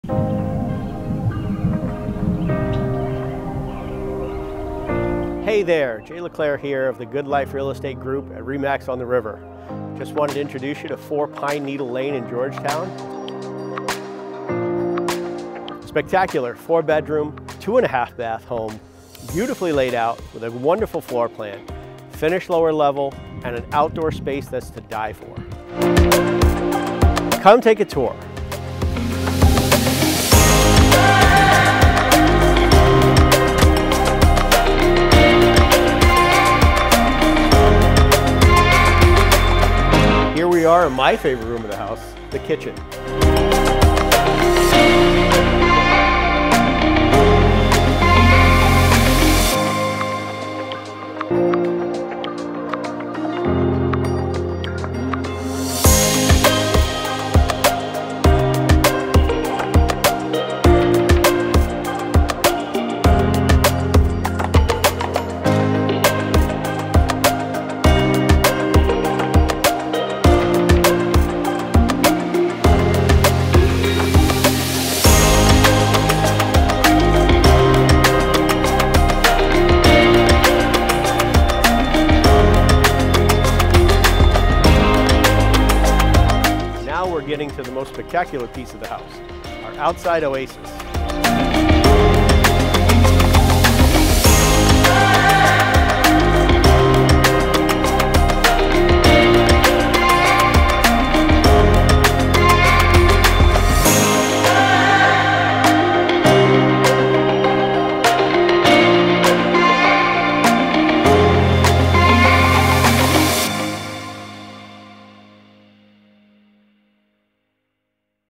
Luxury home north of Boston: Georgetown, MA video tour
When our clients asked for help marketing a house for sale, we knew we had to incorporate a video tour. Look at that backyard and the interior finishes!